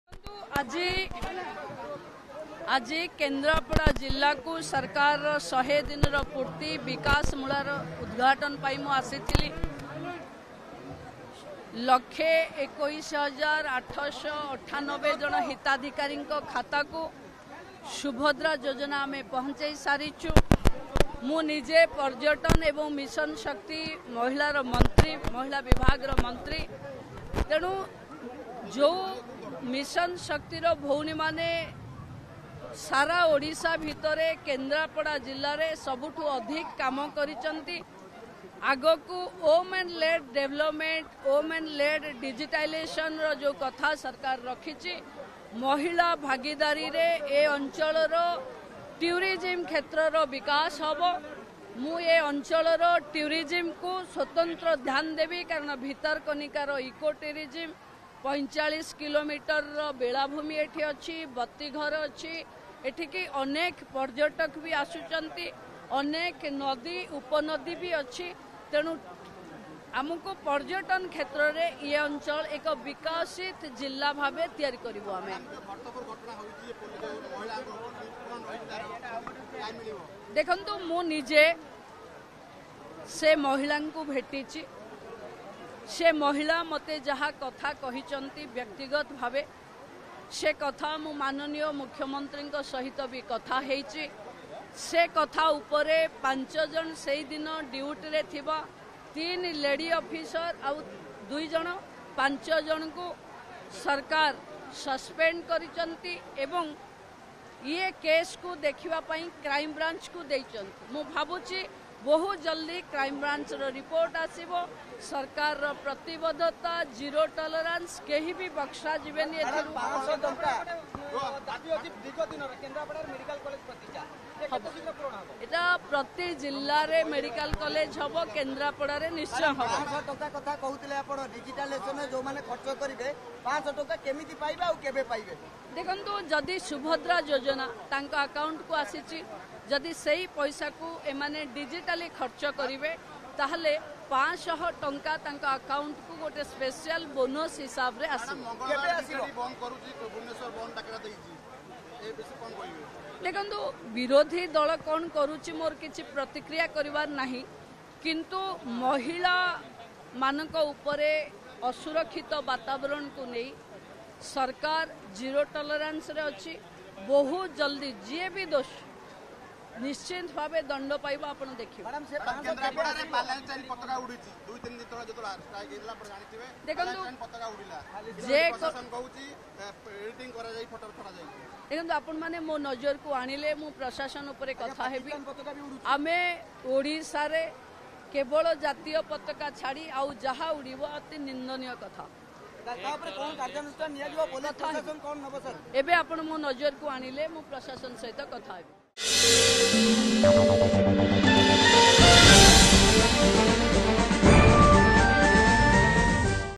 We Will Make Kendrapara Into a Tourism Hub: Odisha Deputy CM Pravati Parida
କେନ୍ଦ୍ରାପଡା ଗସ୍ତରେ ଥିବା ଉପମୁଖ୍ୟମନ୍ତ୍ରୀ ପ୍ରଭାତୀ ପରିଡ଼ା କ'ଣ କହୁଛନ୍ତି ଶୁଣନ୍ତୁ.